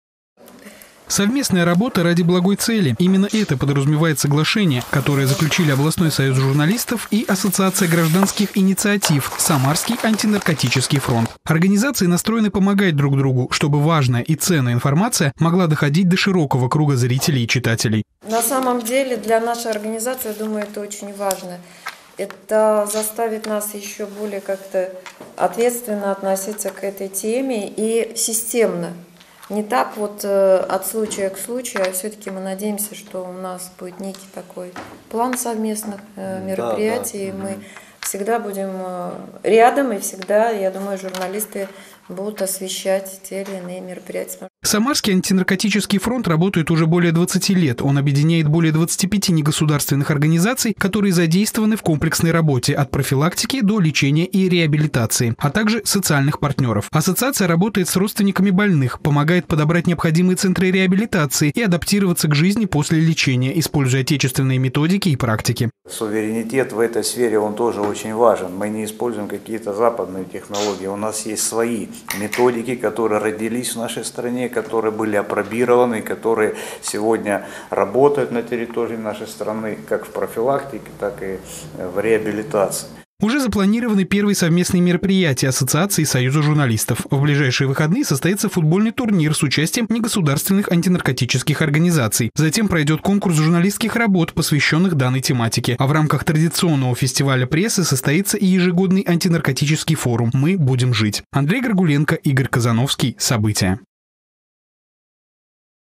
Союз журналистов и "Самарский антинаркотический фронт" заключили соглашение о сотрудничестве
Сила слова в помощь людям. Областной союз журналистов и ассоциация гражданских инициатив "Самарский антинаркотический фронт" подписали соглашение о сотрудничестве. В его рамках на системной основе будут проходить профилактические акции, конкурсы и форумы. Стороны уже наметили ближайшие планы и готовы к регулярной совместной работе. Подробнее в нашем сюжете.